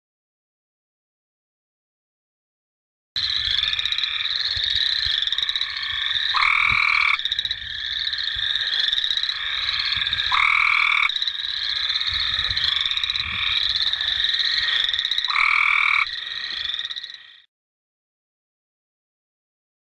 Cope's gray treefrog 1
Cope's gray treefrogs calling in a wetland. Also calling are boreal chorus frogs and American toads. Note: there are no eastern gray treefrogs calling in this recording.